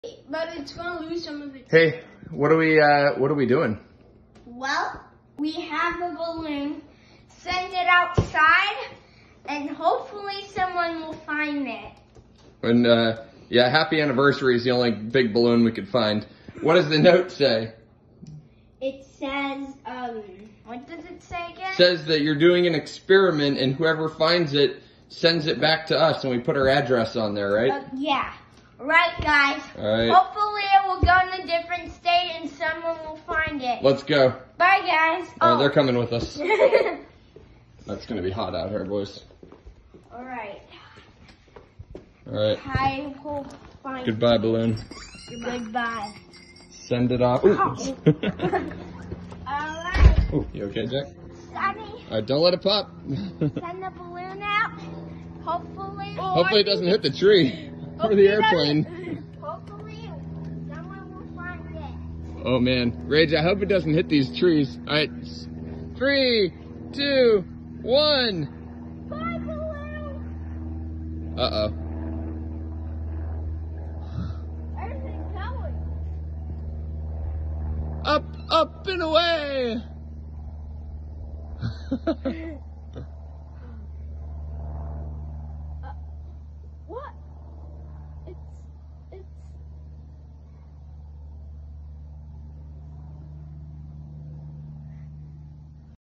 Balloon with a Note
Raylan and Jack launch a balloon with a note. It’s an experiment to see how far a balloon can travel. Dad (Chris) is launch commander.